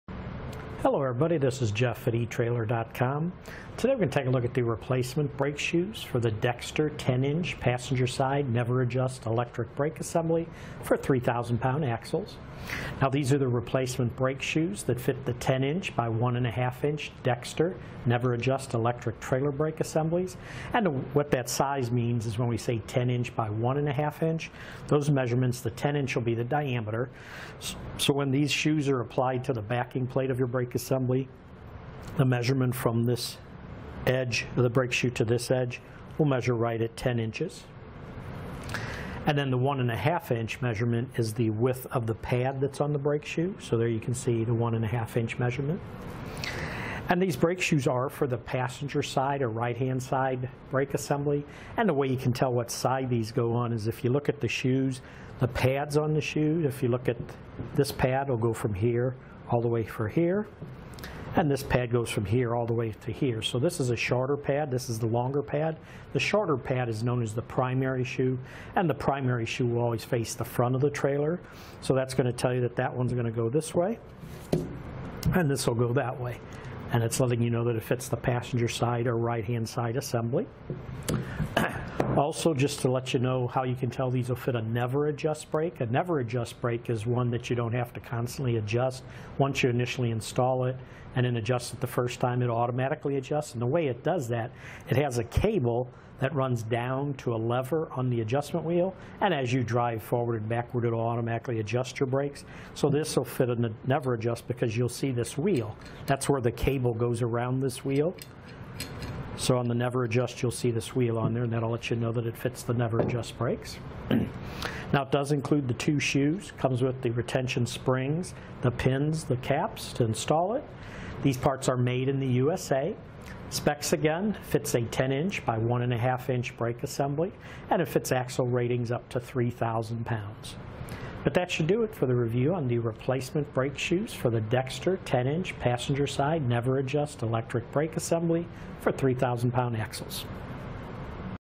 etrailer | Hands-On with the Replacement Brake Shoes for Dexter 10" Nev-R-Adjust Electric Brake
Hello everybody. This is Jeff at etrailer.com. Today, we're going to take a look at the replacement brake shoes for the Dexter 10 inch passenger side, NEV-R-Adjust electric brake assembly for 3000 pound axles.Now, these are the replacement brake shoes that fit the 10 inch by 1 1/2 inch Dexter NEV-R-Adjust electric trailer brake assemblies. And what that size means is when we say 10 inch by 1 1/2 inch, those measurements, the 10 inch will be the diameter. When these shoes are applied to the backing plate of your brake assembly, the measurement from this edge to the brake shoe, to this edge we'll measure right at 10 inches. And then the 1 1/2 inch measurement is the width of the pad that's on the brake shoe.

So there, you can see the 1 1/2 inch measurement, and these brake shoes are for the passenger side or right-hand side brake assembly. And the way you can tell what side these go on is, if you look at the shoes, the pads on the shoe, if you look at this pad will go from here all the way for here. And this pad goes from here all the way through here.This is a shorter pad. This is the longer pad. The shorter pad is known as the primary shoe and the primary shoe will always face the front of the trailer.

So that's going to tell you that, that one's going to go this way and this will go that way. And it's something that it fits the passenger side or right-hand side assembly.Also, just to let you know how you can tell these will fit a NEV-R-Adjust brake, a NEV-R-Adjust brake is one that you don't have to constantly adjust. Once you initially install it and adjust it the first time, it automatically adjusts and the way it does that, it has a cable that runs down to a lever on the adjustment wheel. And as you drive forward and backward, it will automatically adjust your brakes.So this will fit in the NEV-R-Adjust because you'll see this wheel that's where the cable goes around this wheel. On the NEV-R-Adjust, you'll see this wheel on there and that'll let you know that it fits the NEV-R-Adjust brakes.

Now it does include the two shoes comes with the retention springs, the pins, the caps to install it. These parts are made in the USA. Specs again, fits a 10 inch by 1 1/2 inch brake assembly. And it fits axle ratings up to 3000 pounds.But that should do it for the review on the replacement brake shoes for the Dexter 10 inch passenger side NEV-R-Adjust electric brake assembly for 3000 pound axles.